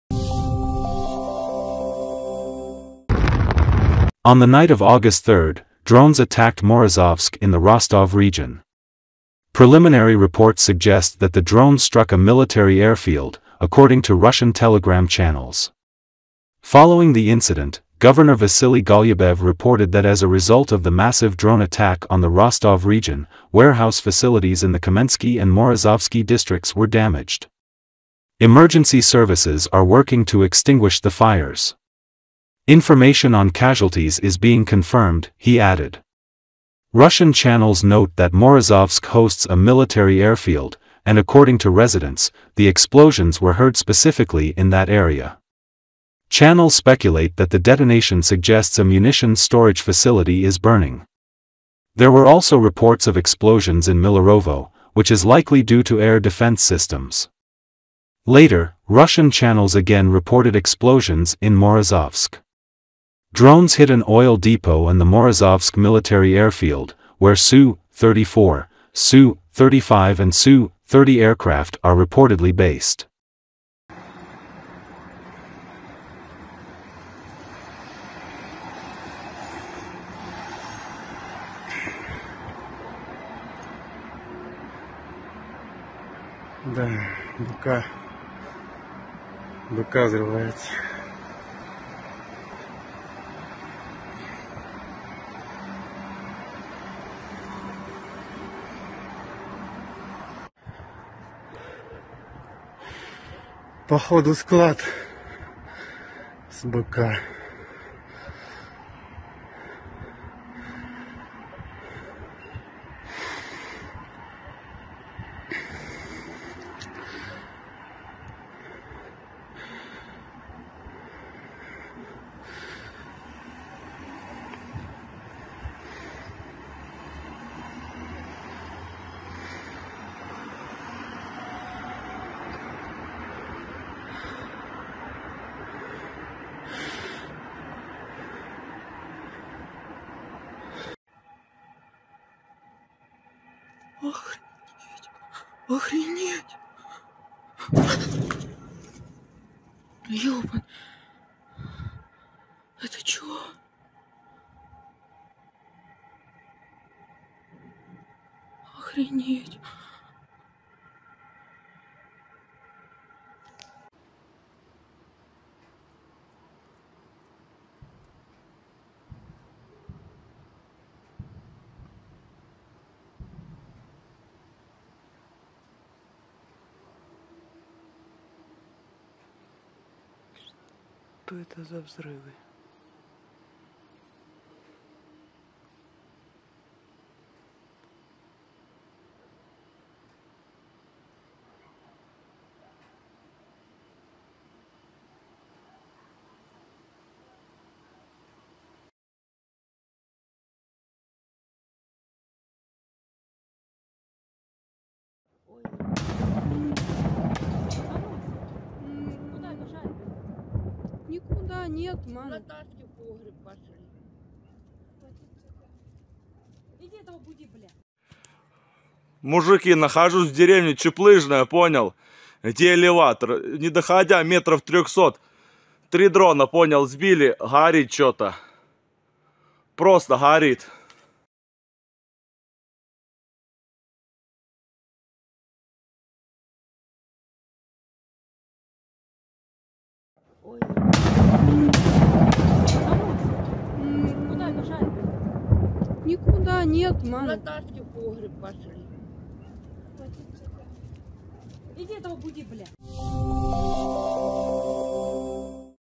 Drones attacked Morozovsk military airfield in Russia's Rostov where fighter jets were stationed
On the night of August 3, drones attacked Morozovsk in the Rostov region. Preliminary reports suggest that the drones struck a military airfield, according to Russian Telegram channels.

Following the incident, Governor Vasily Golubev reported that as a result of the massive drone attack on the Rostov region, warehouse facilities in the Kamensky and Morozovsky districts were damaged.

"Emergency services are working to extinguish the fires. Information on casualties is being confirmed," he added.

Russian channels note that Morozovsk hosts a military airfield, and according to residents, the explosions were heard specifically in that area.

Channels speculate that the detonation suggests a munitions storage facility is burning. There were also reports of explosions in Millerovo, which is likely due to air defense systems.

Later, Russian channels again reported explosions in Morozovsk. Drones hit an oil depot and the Morozovsk military airfield, where Su-34, Su-35 and Su-30 aircraft are reportedly based.